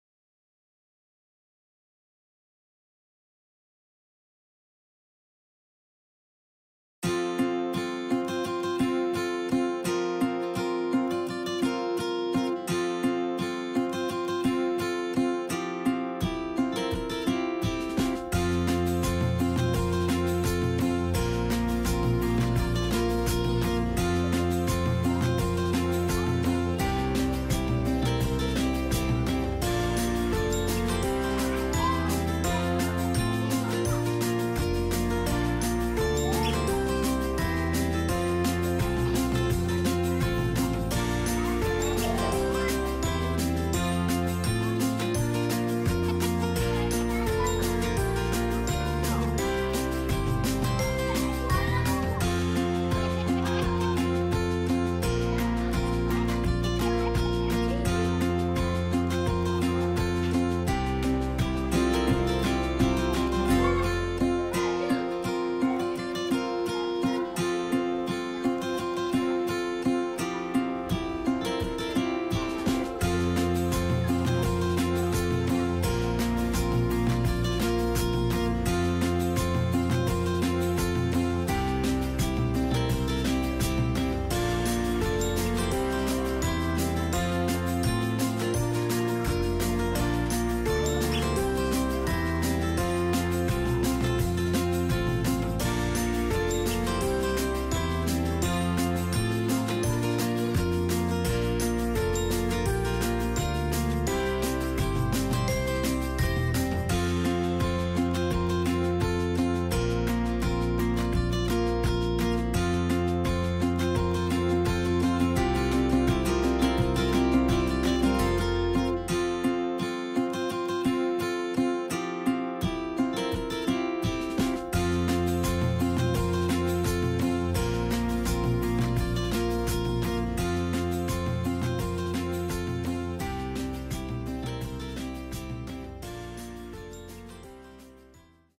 Okulöncesi Öğrencilerimizin Görsel Sanatlar Projesi
Okulöncesi öğrencilerimiz Görsel Sanatlar dersi kapsamında kendilerine verilen aynalarla duygularının yansımalarını yüzlerinde incelediler ve büyük bir beze akrilik boyalarla resimlediler. Enerjileriyle ve coşkularıyla yaşamımızı renklendiren öğrencilerimizin bu çalışması modern insanlarımızın kıyafetlerine dönüştürüldü.

29 Ekim kutlamalarımızın bir parçası haline gelen öğrencilerimizin bu güzel çalışmaları Okul kampüsümüzde sergileniyor.